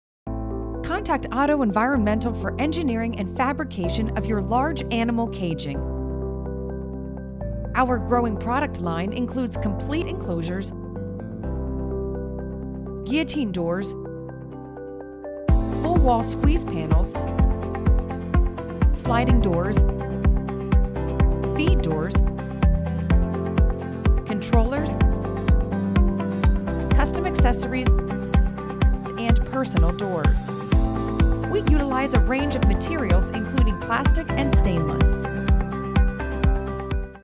Animal Caging, Enclosures, Transport and More by Otto Environmental
Otto Environmental specializes in engineering, fabrication, and installation of your large animal caging. Our cages are fully customizable to your needs and fabricated with stainless steel that is built to last. Our growing product line includes caging, complete enclosures, guillotine doors, sliding doors, feed doors, personnel doors, wall squeeze panels, squeeze caging, mechanical door lifters and controllers, transport caging, animal training aids, animal enrichment products and more.